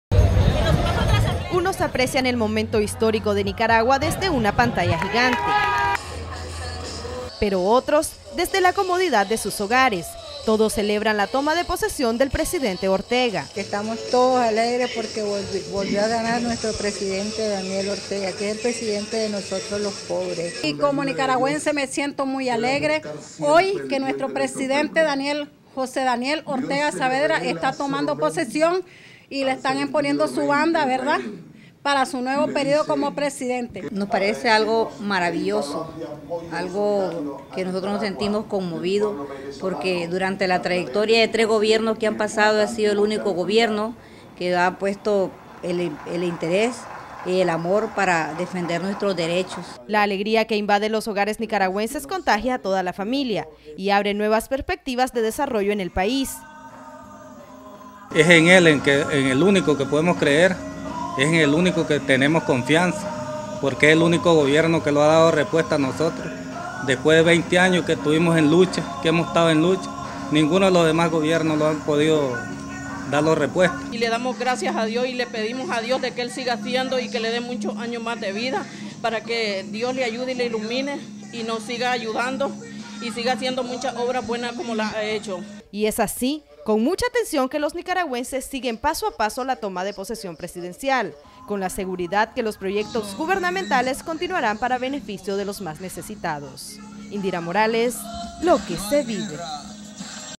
CASAS PARA EL PUEBLO
Desde la Plaza de la revolución, en las avenidas y en las rotondas se siguió  paso a paso la toma de posesión del Presidente y Vicepresidente de la República por elección popular, pero también hay quienes formaron parte del festejo desde su hogar.